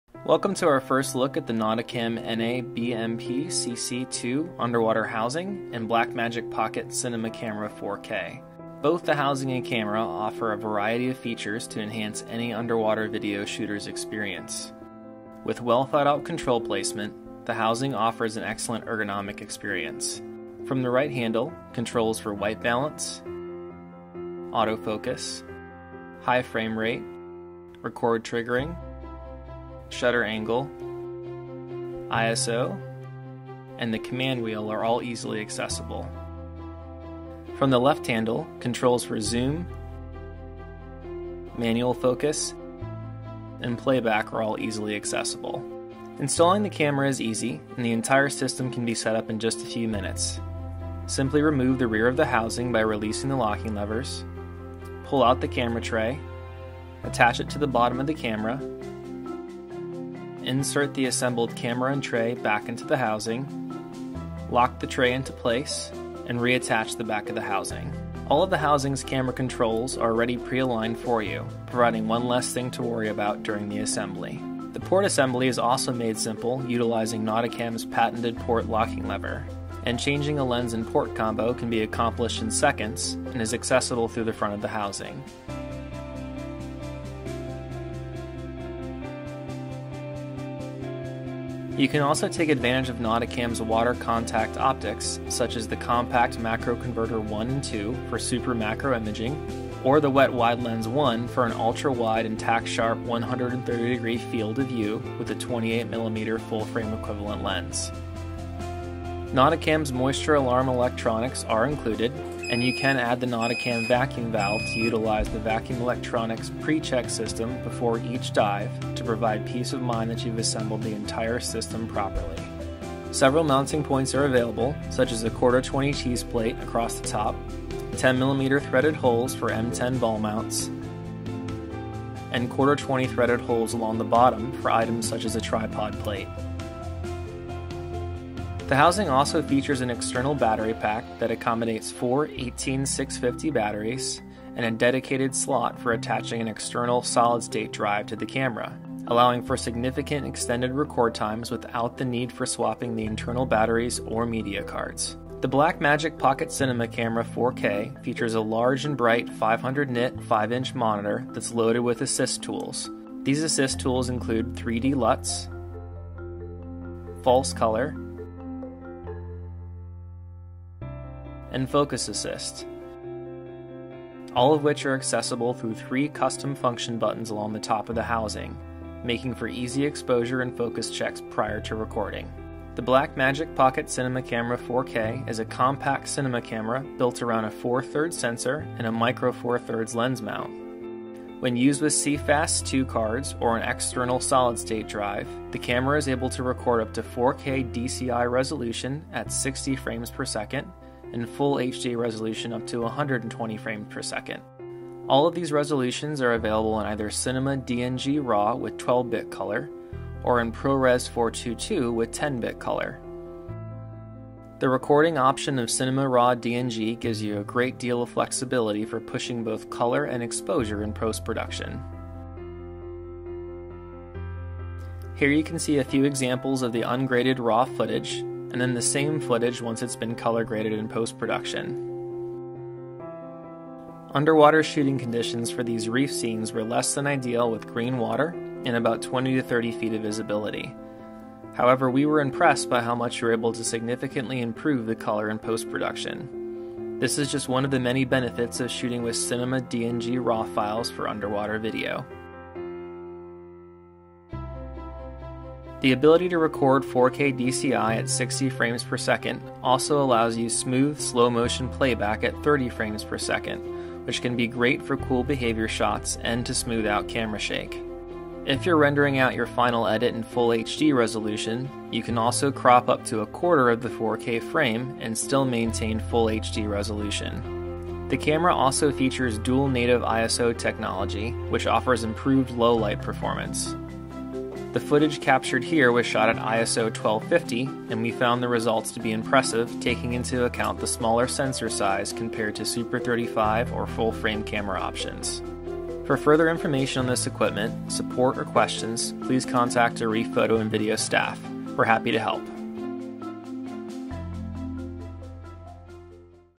First Look Nauticam NA BMPCCII Underwater Housing and Blackmagic Pocket Cinema Camera 4K
The Nauticam NA-BMPCCII underwater housing and Blackmagic Pocket Cinema Camera 4K have a whole variety of features to enhance your underwater shooting experience. This comprehensive review will cover all of the features of the camera and housing, and demonstrate some of the benefits for shooting with this system for underwater.

Have questions or need further assistance on the right underwater equipment for you?